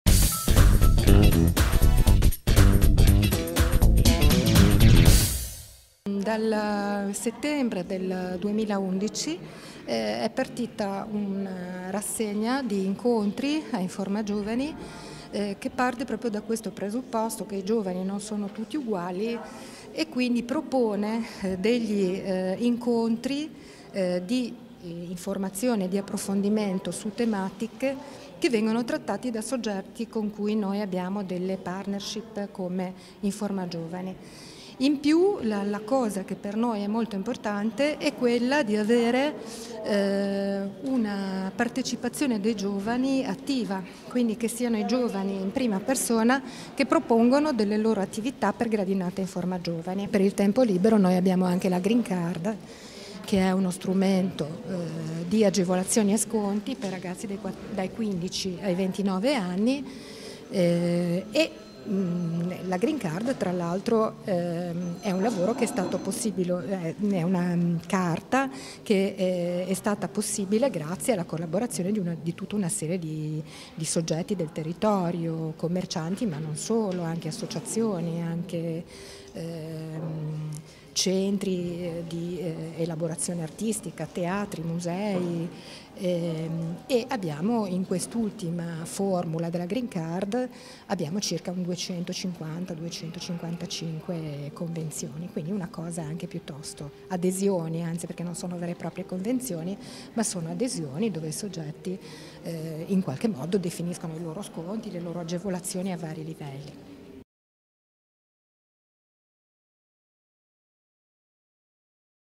Gradinata Informagiovani e la Green Card
Il progetto di Informagiovani Genova per i giovani protagonisti e la Green Card che permette numerose agevolazioni sul territorio